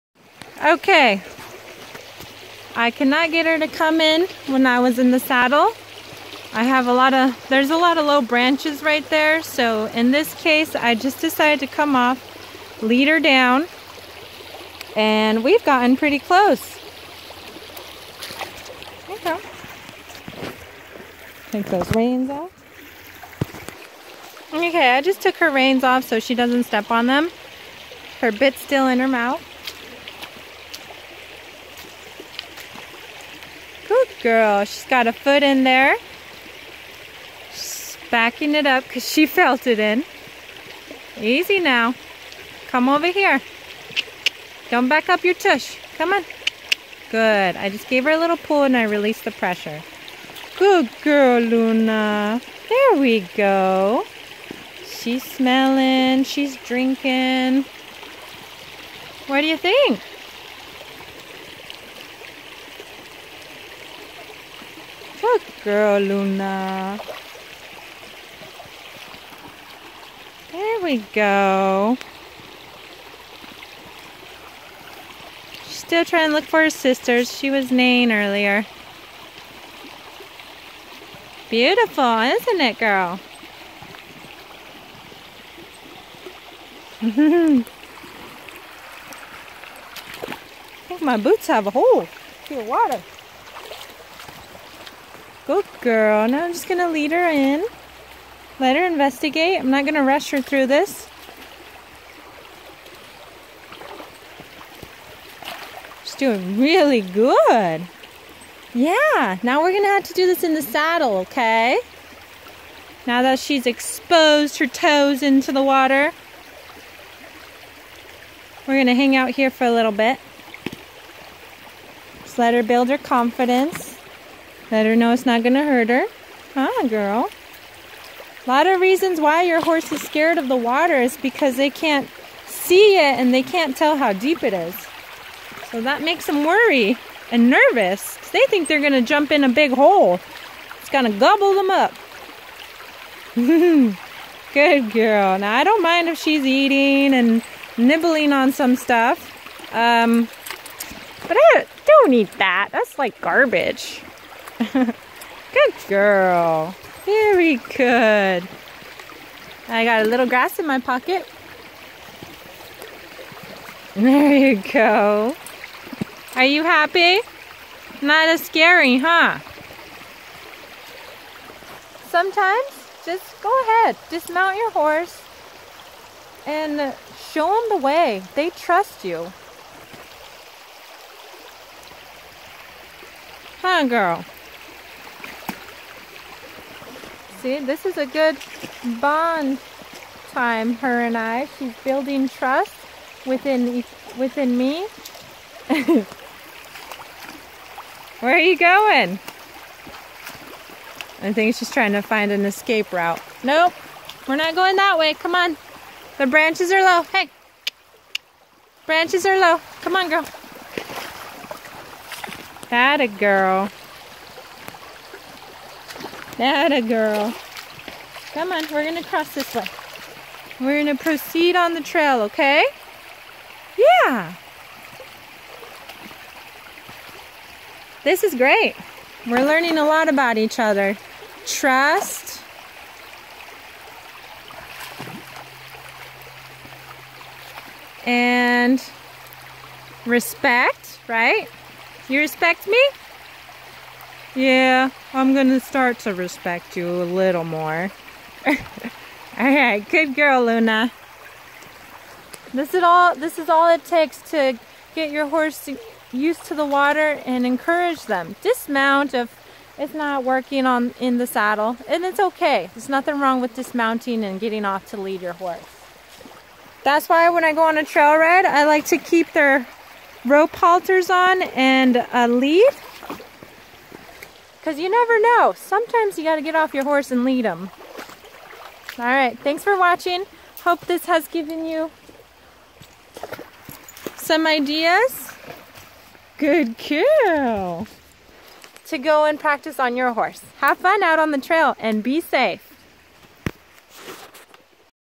ME Horsemanship - How to build confidence to cross the creek pt2
I dismounted to guide her into the creek and help her build trust. I noticed immediately Luna walked in with confidence and started to trust me. That was a great feeling to know that your horse trusts you. This also went smooth from spending time in the saddle with consistent pressure and release before I dismounted. Horses, Ranchin, Family Horses, Ranchin, Family